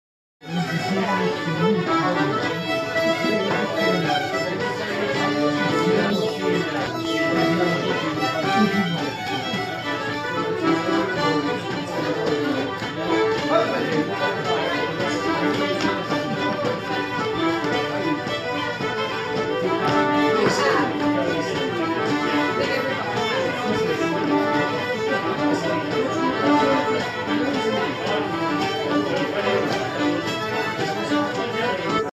Jam session at Shortt's Bar and Lounge in Feakle, Co. Claire, Ireland
Thursday night jam session